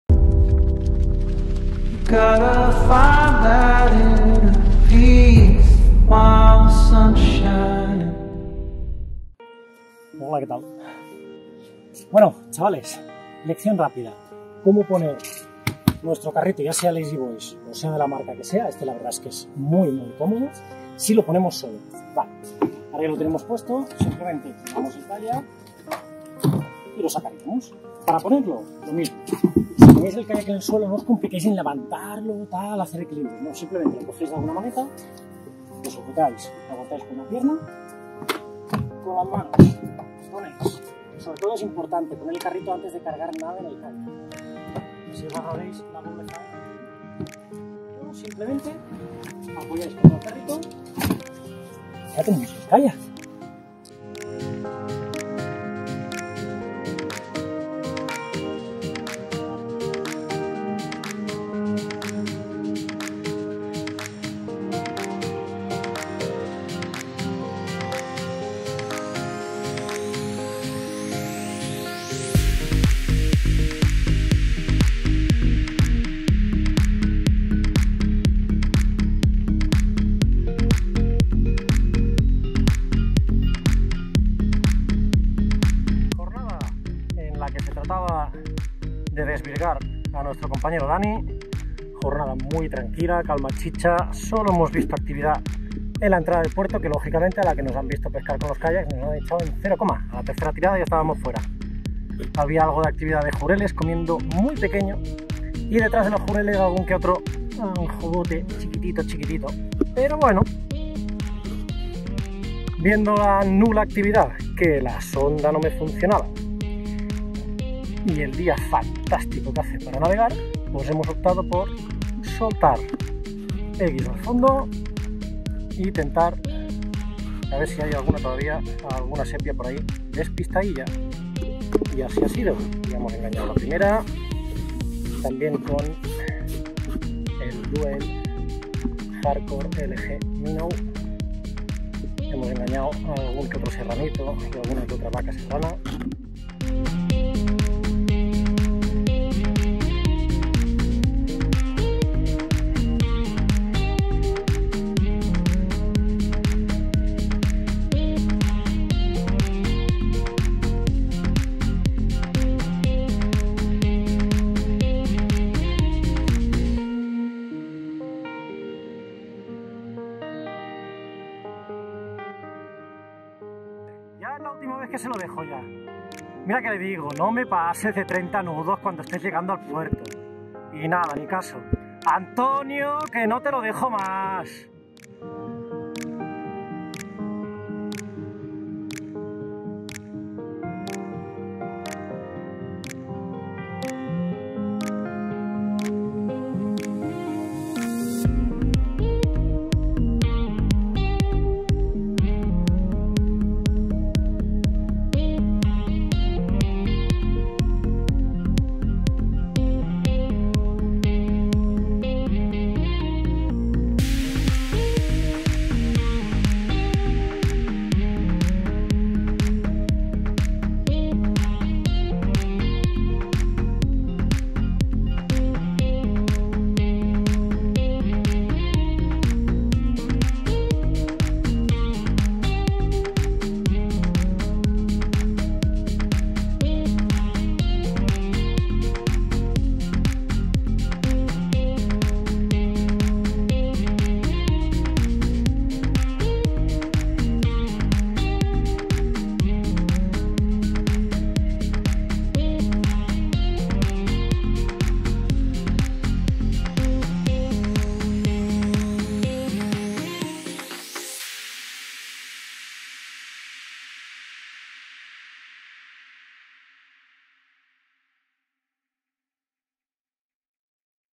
Joan Del Team Galaxy España enseñando a poner carro en su kayak y salida con novato
Joan del Team Galaxy España nos muestra cómo poner un carrito de kayak en su kayak y varios consejos para principiantes.